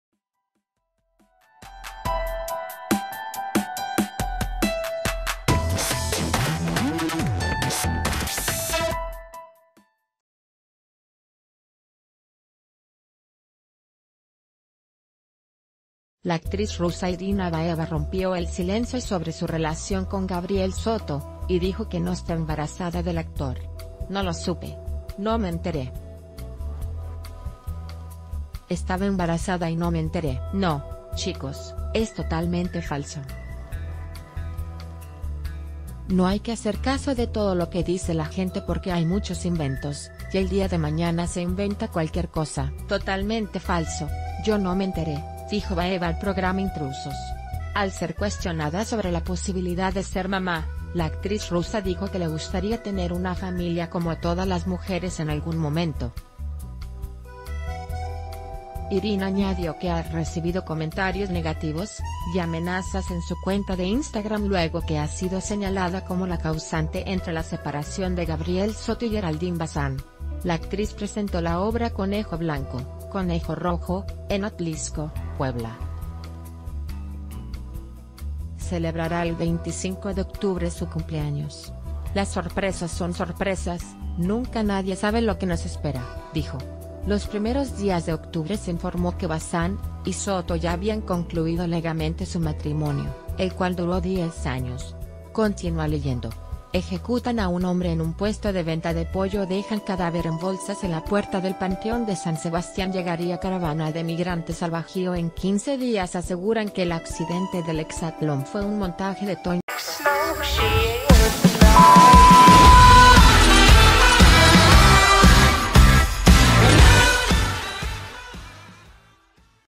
Irina Baeva declara que no está embarazada de Gabriel Soto
La actriz rusa Irina Baeva rompió el silencio sobre su relación con Gabriel Soto y dijo que no está embarazada del actor.  "No lo supe. No me enteré. Estaba embarazada y no me enteré. No, chicos, es totalmente falso. No hay que hacer caso de todo lo que dice la gente porque hay muchos inventos y el ...